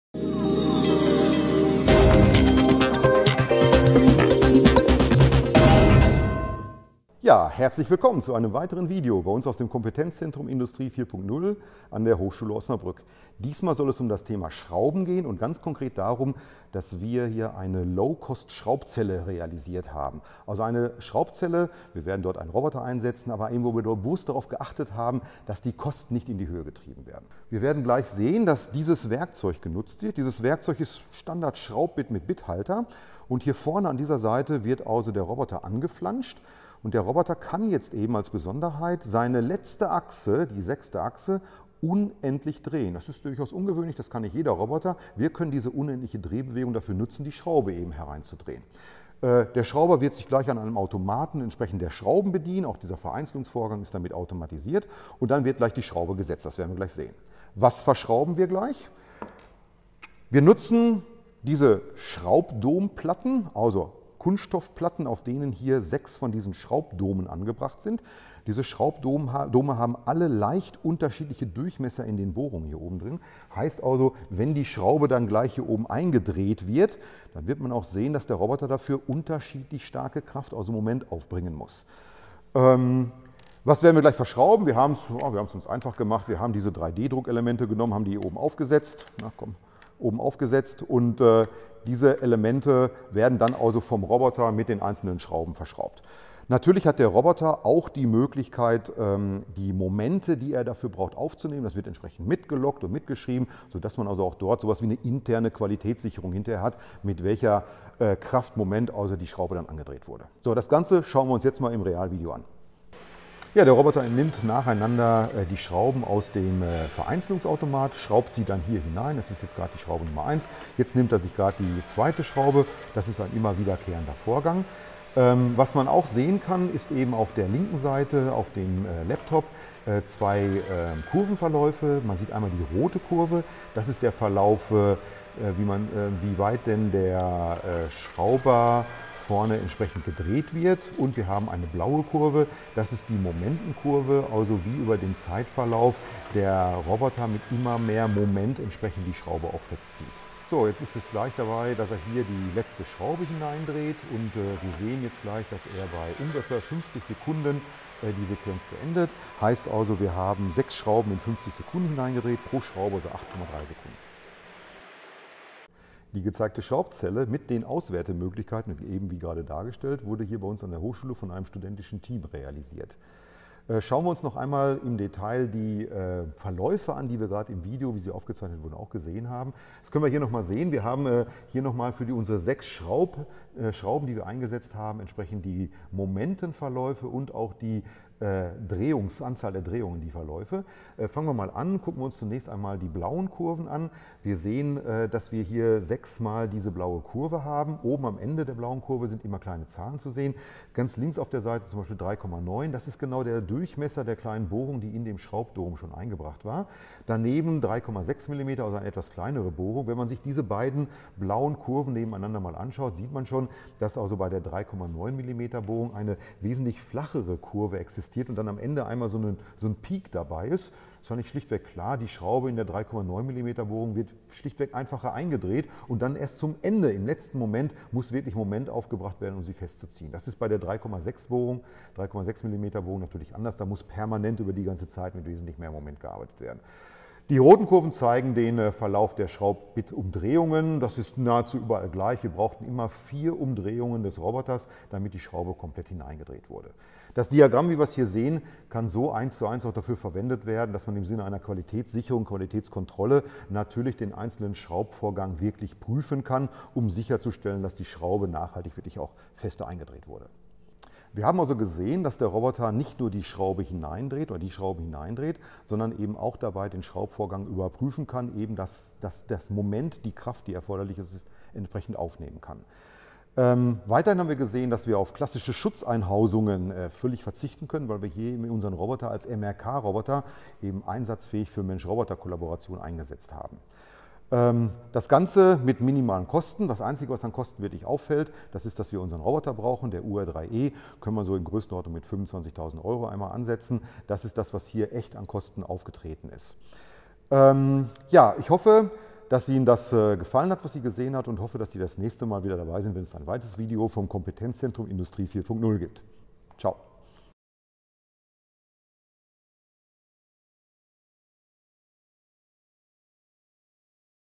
Realisierung einer LowCost-Schraubzelle mit Mensch-Roboter-Kollaboration
Drei studentische Wissenschaftler der Hochschule Osnabrück haben es sich in einer Studienarbeit zur Aufgabe gemacht den Prozess des Schraubens zu optimieren. Ziel war es dabei industrielle Schraubvorgänge unter Einsatz der Mensch-Roboter-Kollaboration mit minimalen Investitionen zu ermöglichen. Im Video zeigen wir die Grundlagen zum Schrauben, die Besonderheiten des Roboters und die Auswertungs- und Qualitätskontrollmöglichkeiten der Mensch-Roboter-Zelle.
Den besonderen "Dreh" hat im Video der UR3e-Roboter: Er ist standardmäßig mit einer endlos drehenden 6. Roboterachse ausgestattet. An der potenitell endlos drehenden Flanschplatte des Roboters ist direkt ein magnetisches Schrauberbit befestigt. Der Roboter pickt damit die von einem Schraubenvereinzeler bereitgestellten Schrauben, positioniert sie am zu verschraubenden Objekt und führt den Einschraubvorgang durch. Dabei werden relevante Daten (Einschraubumdrehungen und das aufzubringende Einschraubmoment) durch den Roboter erfasst, gespeichert und ausgewertet. Pro Schraube liegt die Zykluszeit bei ca. 8 Sekunden. Als MRK-Zelle kann auf klassische Schutzeinrichtungen verzichtet werden, womit ebenfalls die Investitionen gering gehalten werden.